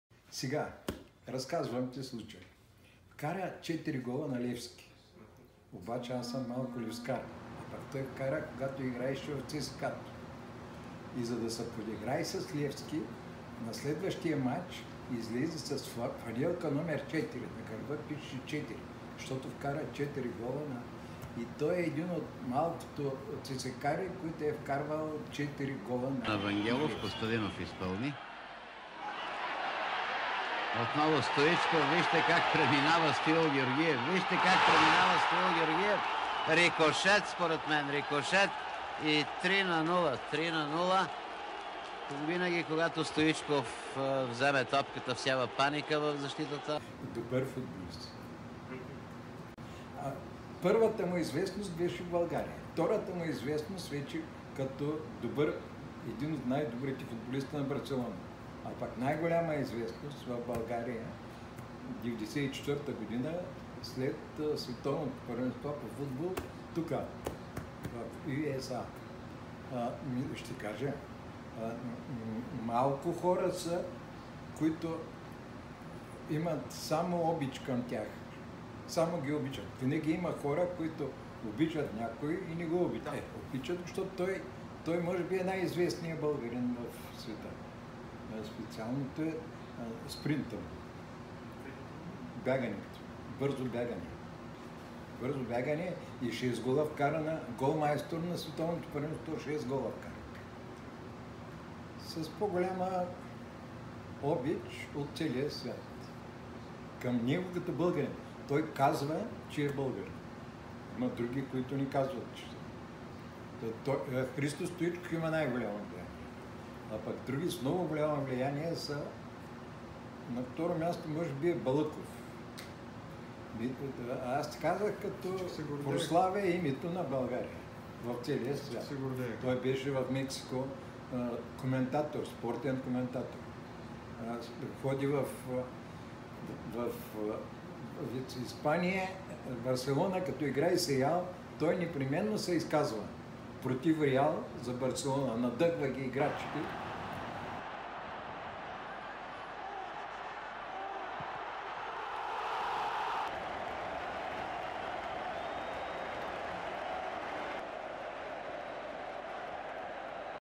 Hristo Stoichkov from the eyes of a Bulgarian. Hristo Stoichkov Mini Documentary
Hristo Stoichkov from the eyes of a Bulgarian. A little about Stoichkov

told from my grandpa
for a school project in 2020